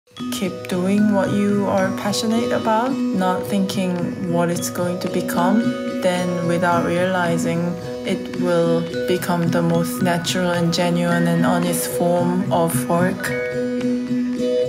Our co-lab with REJINA PYO is here | & Other Stories
We’re proud to present our limited co-lab collection with Rejina Pyo. The Korean-born, London-based fashion designer known for her signature dresses’ sculptural beauty and relaxed suiting with unexpected twists that encourage individual instincts. Discover the collection at stories.com.